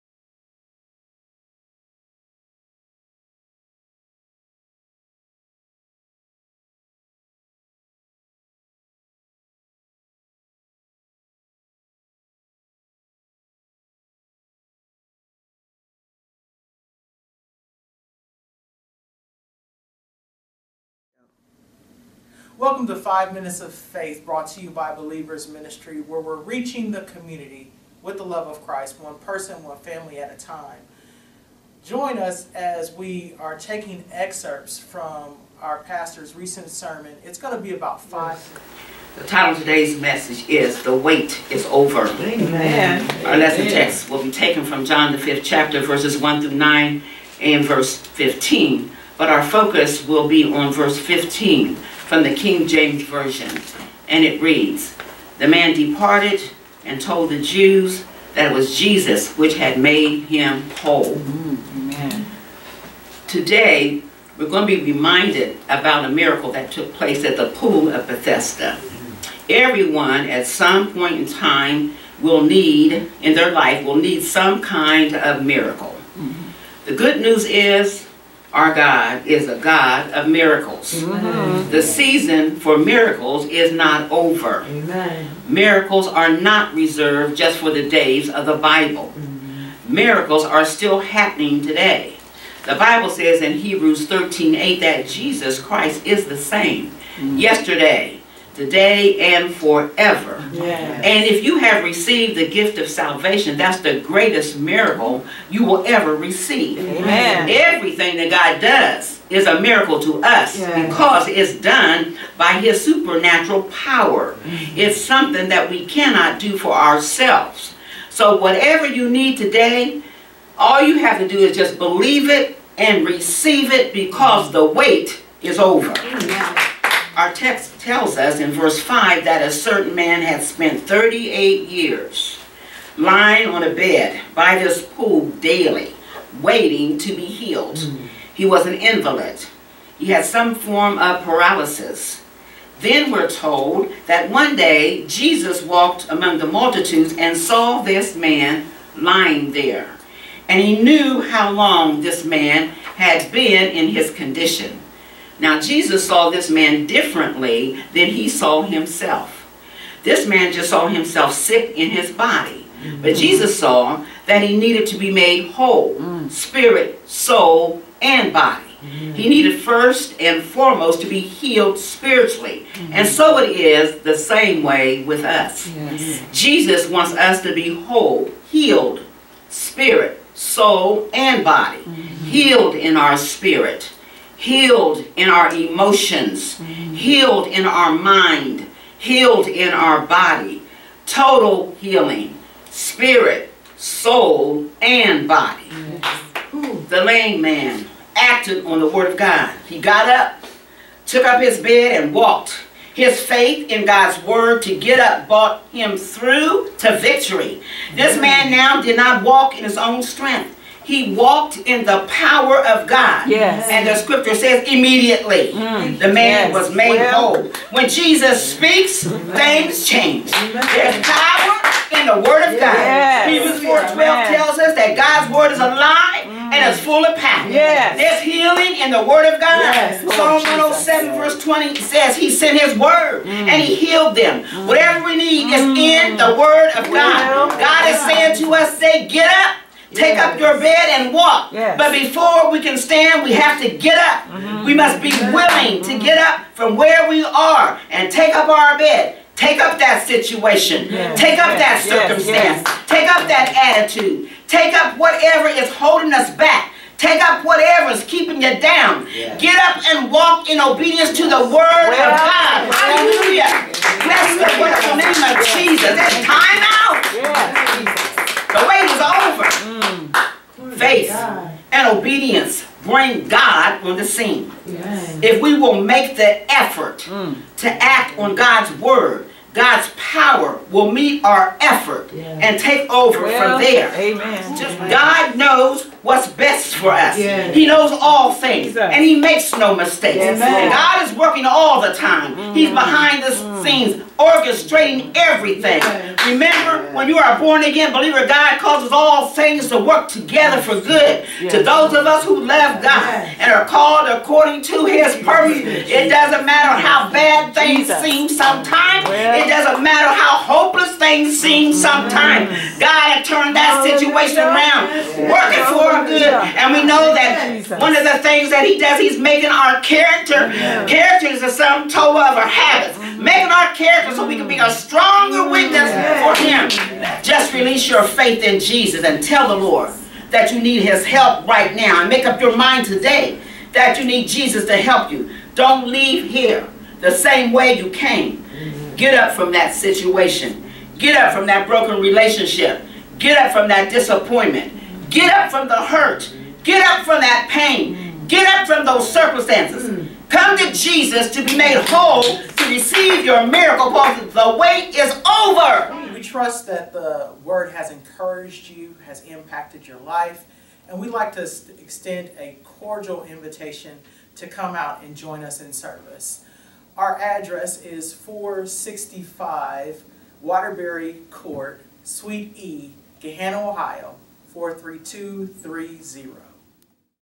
5 Minutes of Faith 18 05 06
About five minutes from the sermon "The Wait Is Over" preached Sunday, April 29, 2018 at Believers' Ministries, 465 Waterbury Court Suite E, Gahanna, OH 43230.